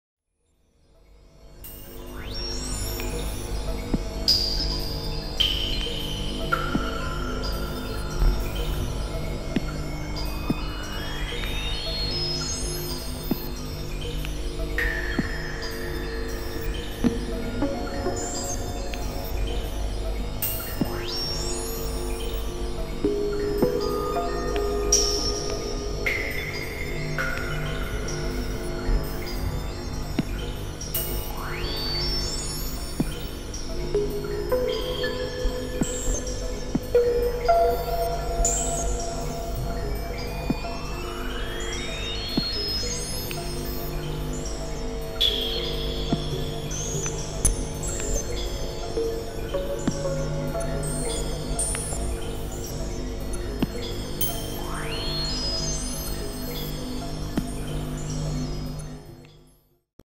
Generative Patch on Behringer Neutron
A generative patch made with Behringer Neutron which was routed into VCV rack for the Plateau Reverb.
The video was made with touchdesigner and lightworks.